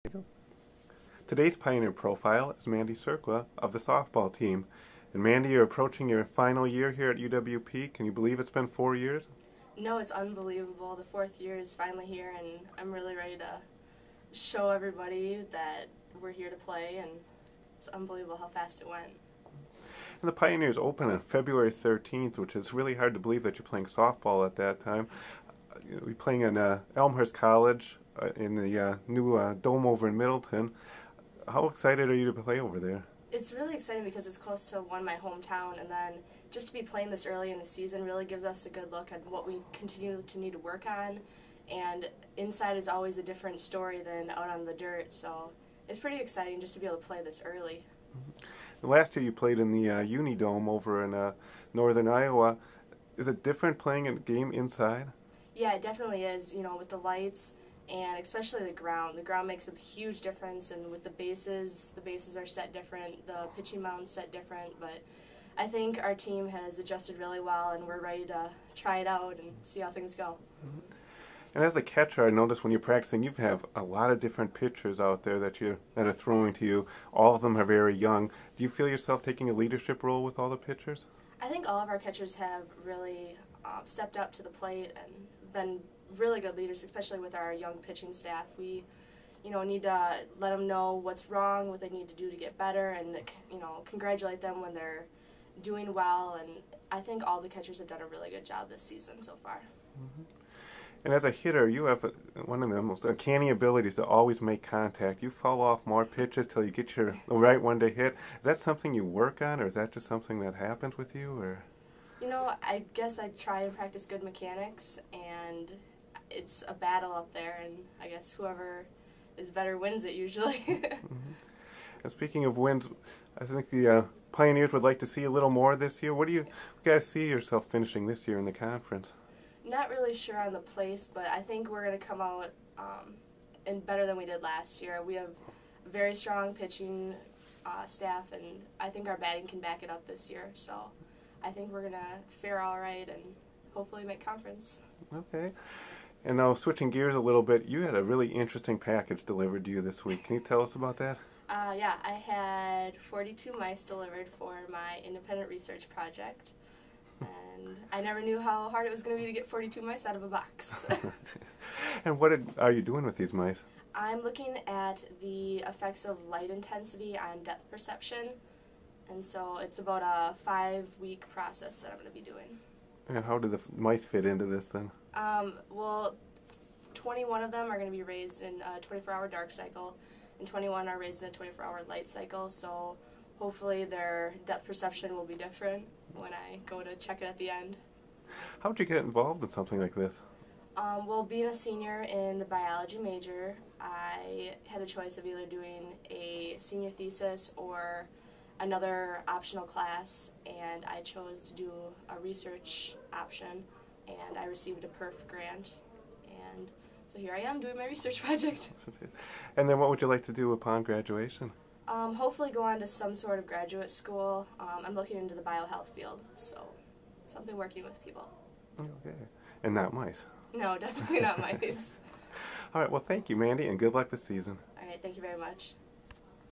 UW-Platteville Softball - Mandy Cerqua (2 5 2010).MP4
UW-Platteville softball player Mandy Cerqua talks about her senior season -- and why she just had 42 mice delivered to her.